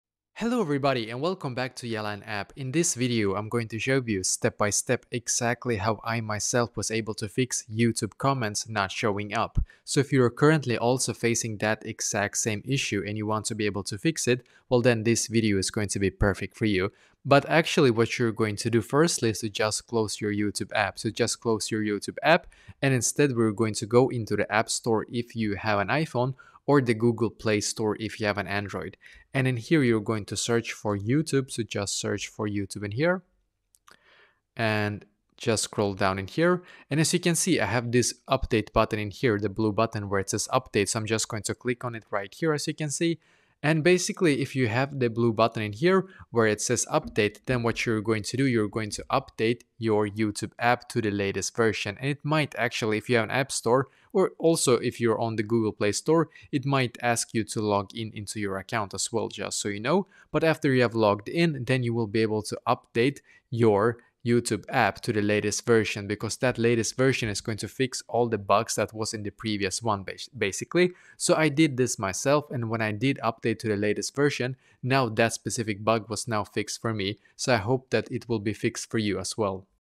How To Fix YouTube Comments Not Showing Up (2025)
If you want to be able to fix youtube comments not showing up, then this video will be perfect for you!